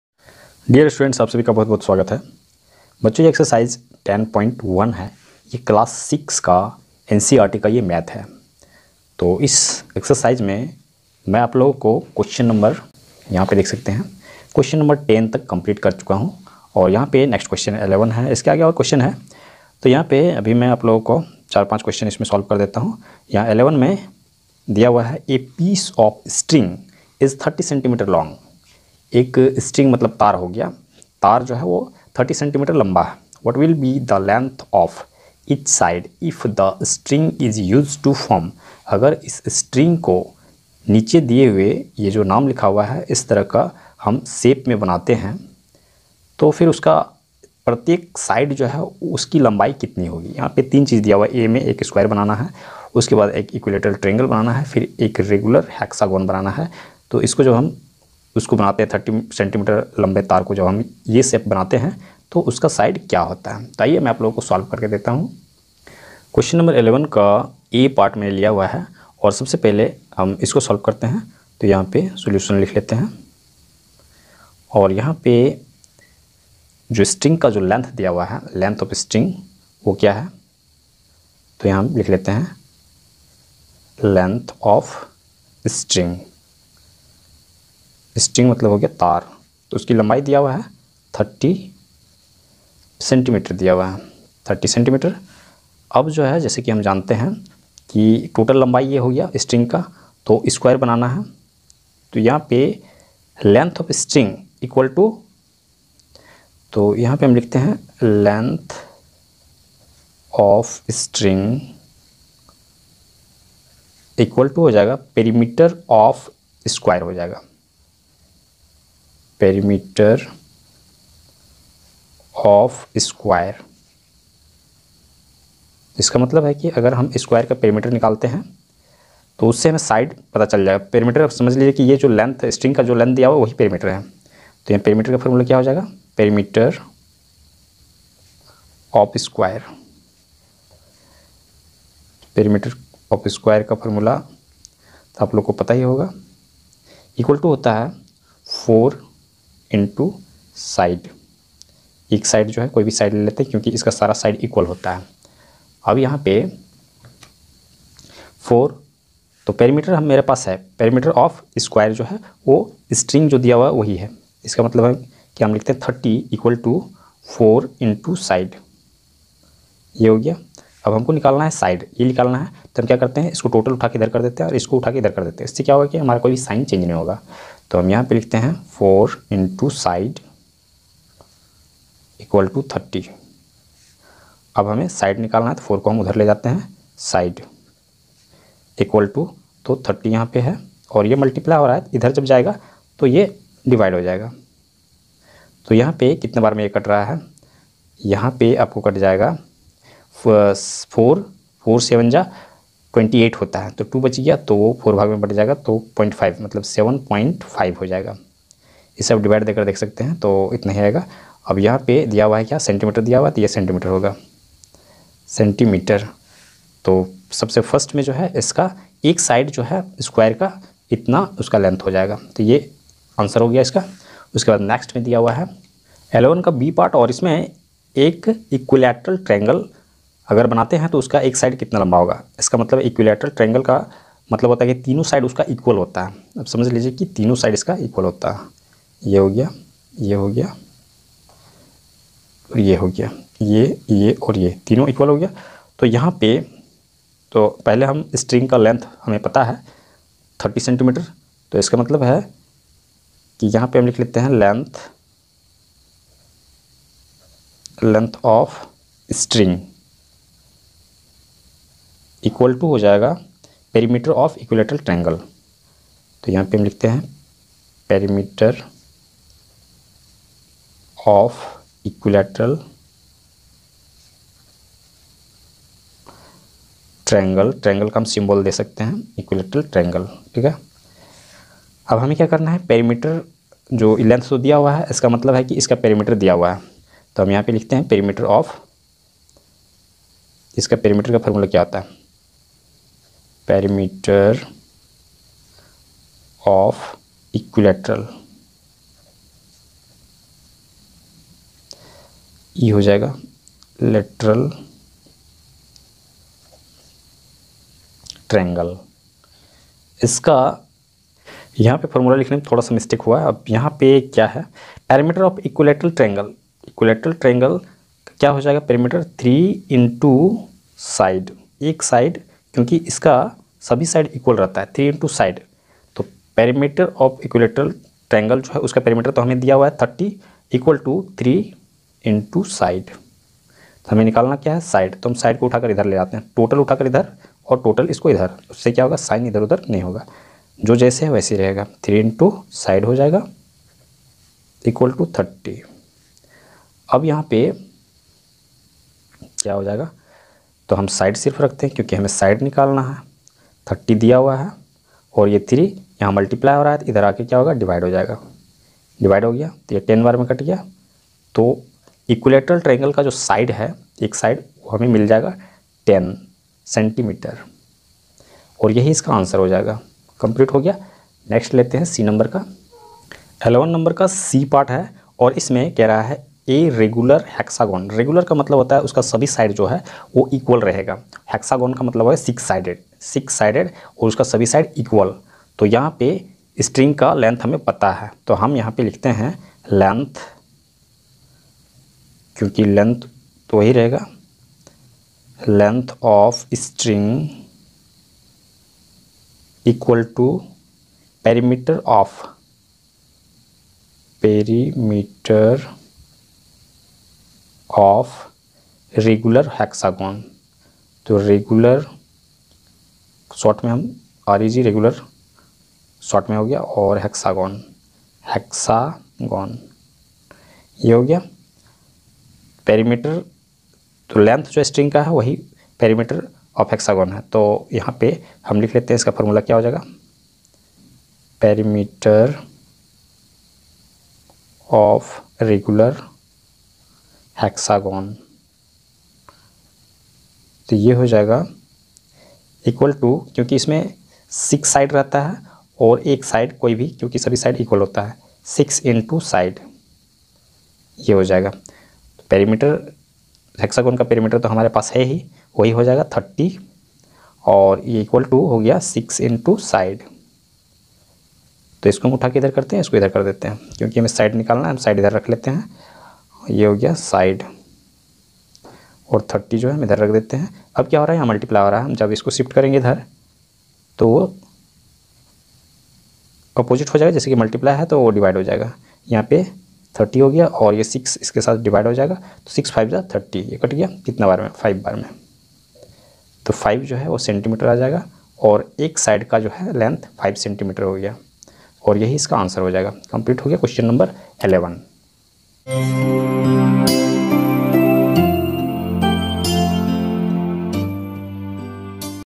Q 11 Ex 10.1 Ncert Maths Solution Class 6 Ch 10 Mensuration Saraswati Shishu Vidya Mandir
नमस्कार
बच्चों इस विडियो में Class 6 Ncert Maths Ex 10.1 Q 11 को Solve किया गया है। आप सभी इस विडियो को ध्यान पूर्वक देखकर समझें और उसे Revise भी करें। धन्यवाद।/